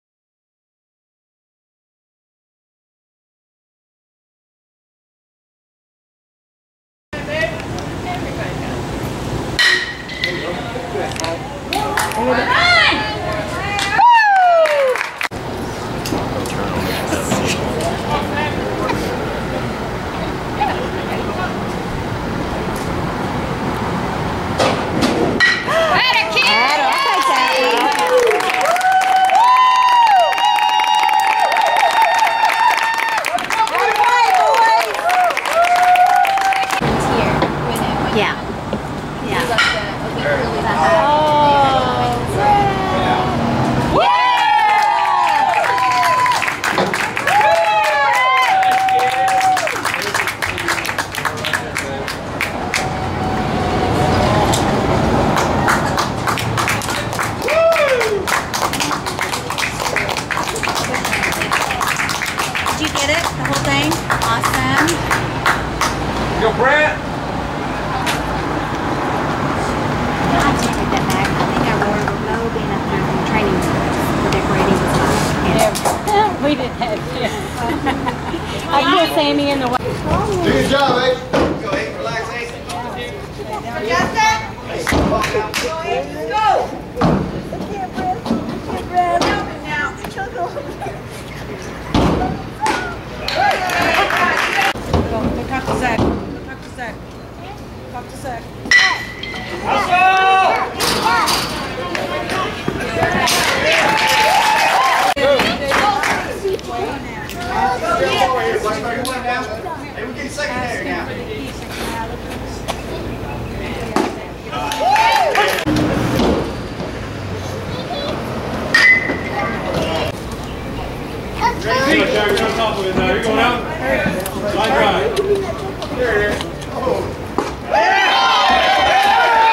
Bret Boswell SS #17 DBAT Mustangs-Perry 2-3 July 2011 -HR-dbl-pitching.wmv
Bret Boswell SS #17, 3 run HR, RBI double, pitching in Championship game at McKinney TX on 3 July 2011.
2013 Rockwall Heath HS, Heath, TX